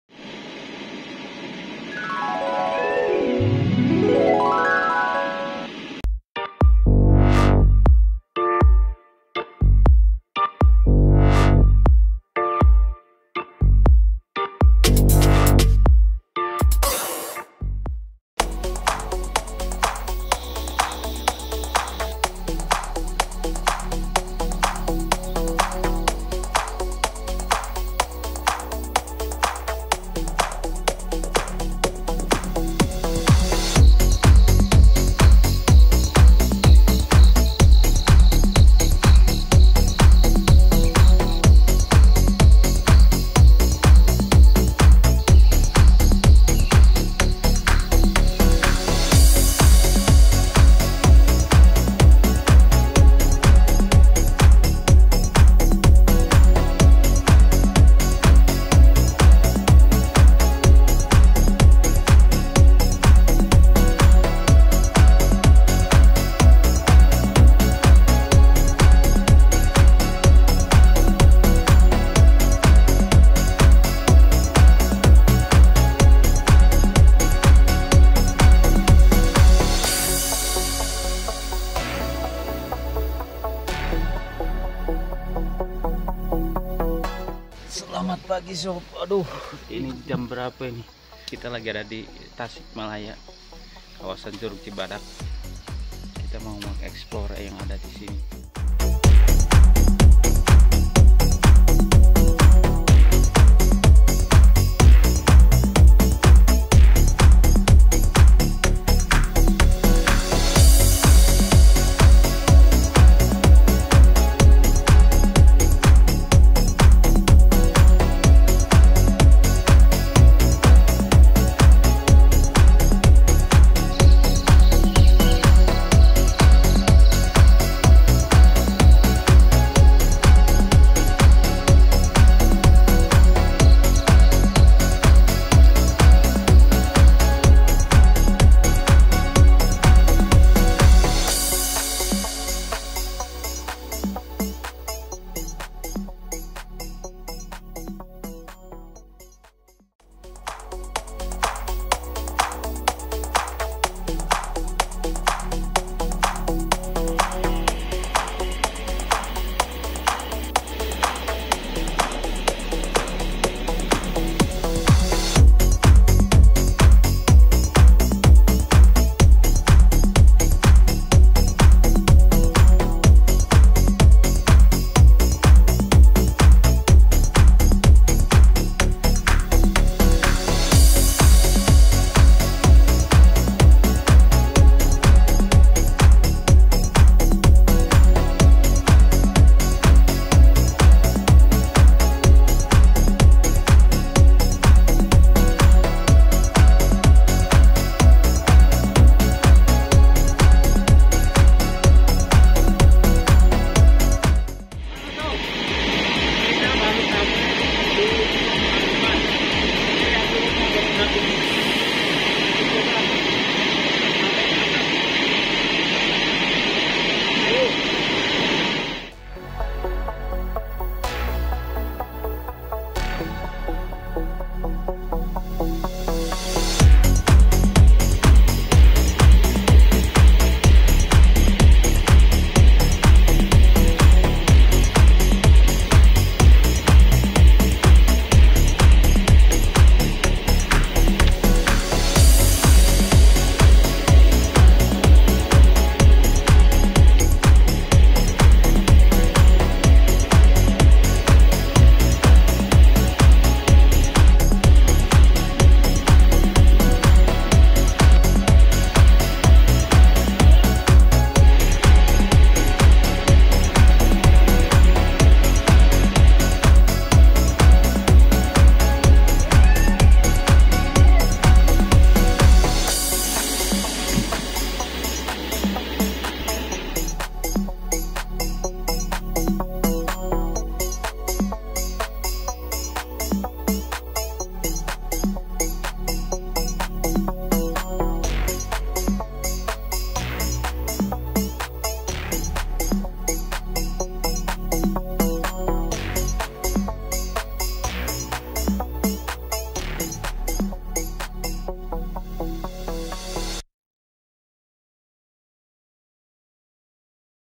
CURUG BATU HANOMAN - CAMPING ASIK DI CIPASUNDAAN
Curug Batu Hanoman, berada di Sukasetia, Kecamatan Cisayong, Kabupaten Tasikmalaya. Air terjun ini berlokasi di area Curug Badak, atau sekira 50 meter di atas Curug Badak. Tiket masuk Rp10.000,00/orang sudah termasuk tiket masuk Curug Badak. Parkir Rp5.000,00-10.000,00. Selain curug, di area ini terdapat banyak warung, cafe, dan camping ground. Salah satunya cafe dan camping ground Cipasundaan. Mari berbagi keindahan. Hayu piknik, tong poho balik.